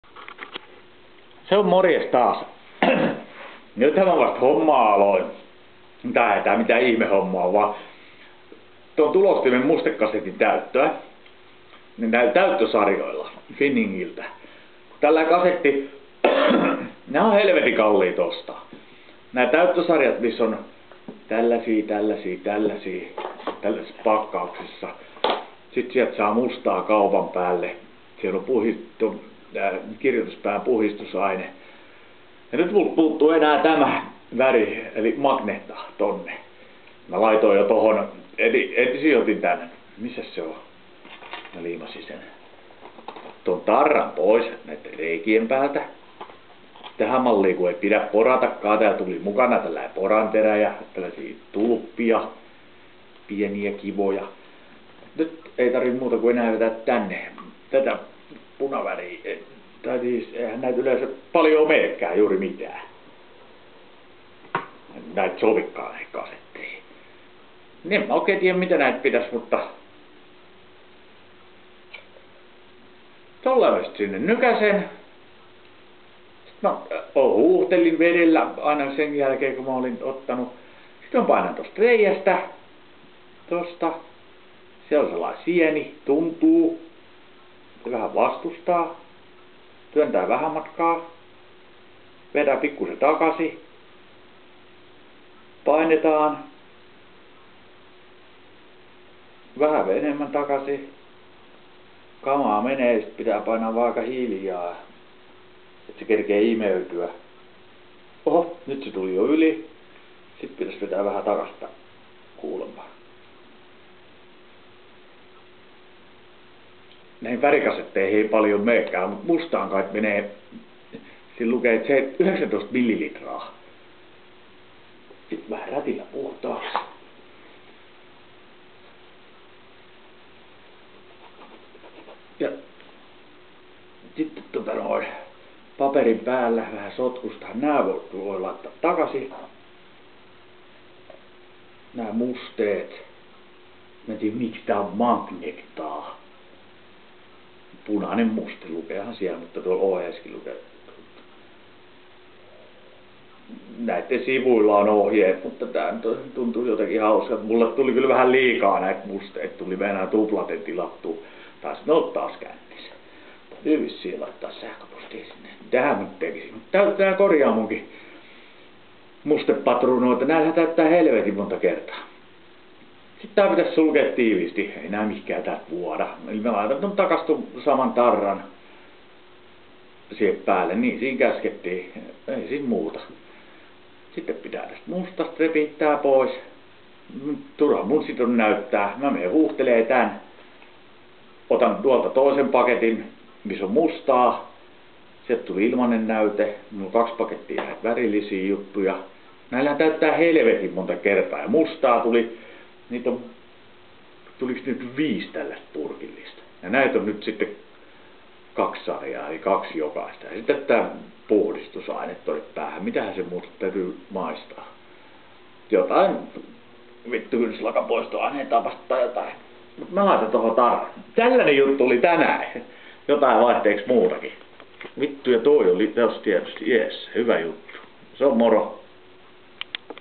Tulostimen mustekasettien täyttösarja.
Oli ihan pakko itse koittaa täyttää mustekasetit ja onnistu hienosti.